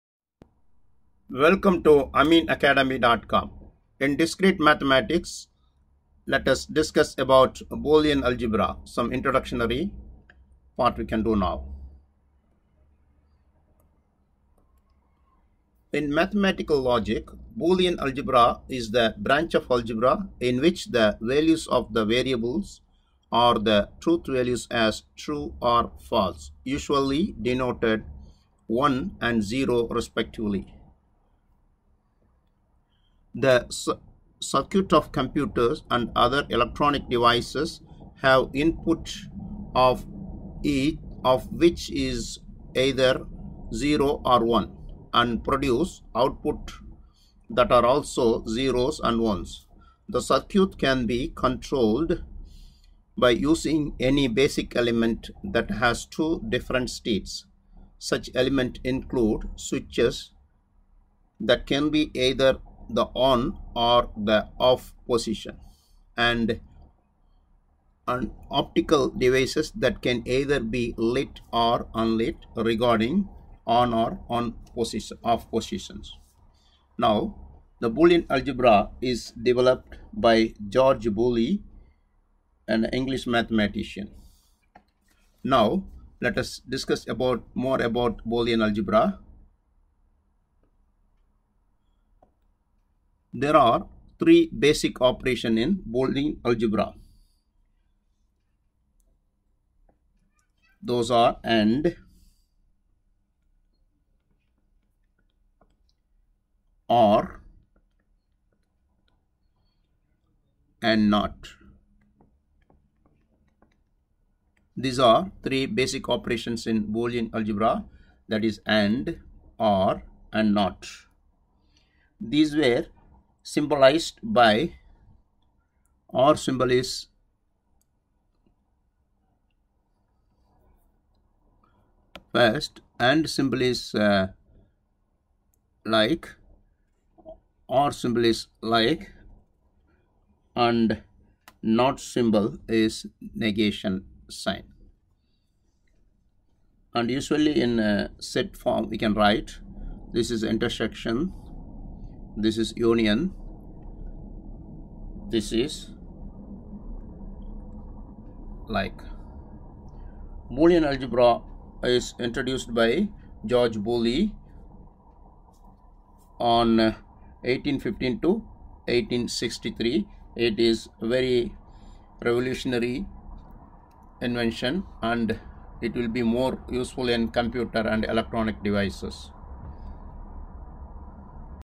Discrete mathematics : - ( Boolean Algebra ; Introduction ) - 61.
Discrete mathematics is the study of mathematical structures that are fundamentally discrete rather than continuous.
Boolean Algebra : In mathematics and mathematical logic , Boolean algebra is the branch of algebra in which the values of the variables are the truth values as true and false , usually denoted 1 and 0 respectively.